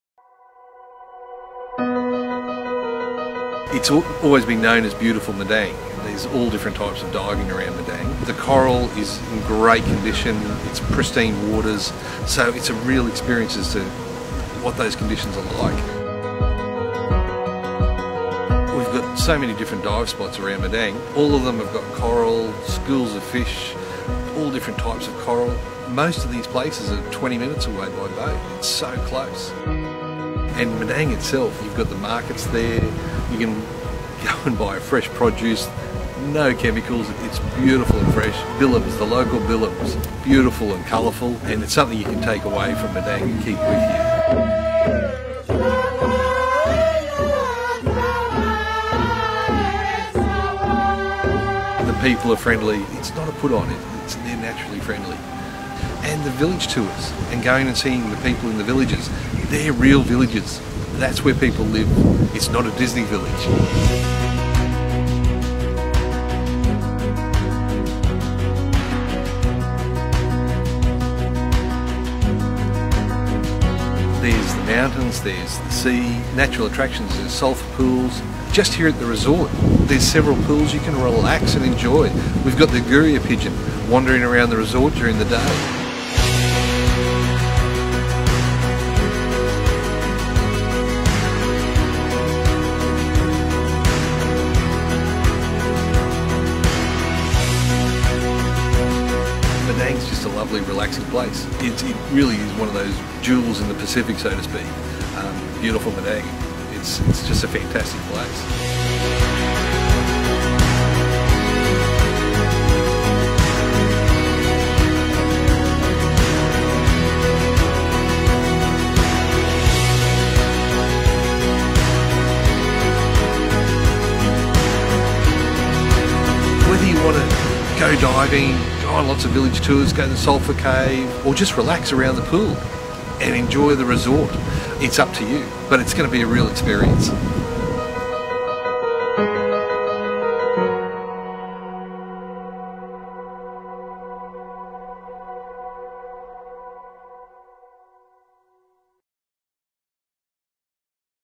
Beautiful Madang
Madang has always been referred to as 'Beautiful'.  This lovely and relaxing place has several natural attractions that are well worth a visit - plus the diving is absolutely fantastic.  The local tours are thoroughly enjoyable - visit the fresh food and craft markets, meet the friendly Madang locals and experience a cultural dance at a nearby village.   Journey Madang